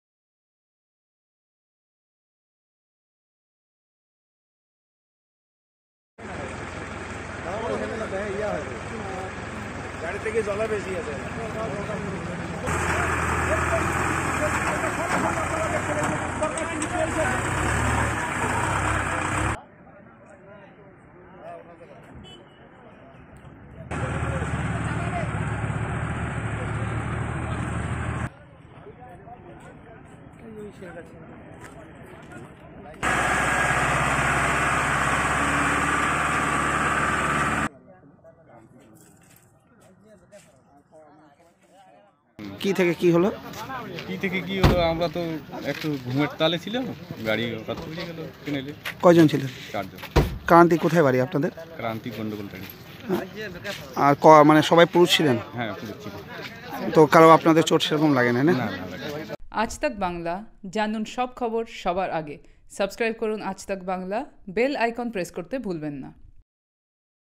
North Bengal News| উত্তরবঙ্গ মেডিকেলে যাওয়ার পথে তিস্তা ক্যানেলে উল্টে গেল গাড়ি
North Bengal News| ক্রান্তি থেকে শিলিগুড়ি আসার পথে একটি ছোট চারচাকা গাড়ি নিয়ন্ত্রণ হারিয়ে তিস্তা ক্যানেলে উল্টে যায়। ঘটনায় ভাগ্যের জোরে প্রাণে বাঁচে গাড়িতে থাকা ৪ ব্যক্তি। খবর পেয়ে ঘটনাস্থলে নিউ জলপাইগুড়ি ও আমবাড়ি থানার পুলিশ কর্মীরা পৌঁছে গাড়িটিকে ক্রেনের সাহায্যে উদ্ধার করে। ঘটনায় চাঞ্চল্য ছড়ায় এলাকায়।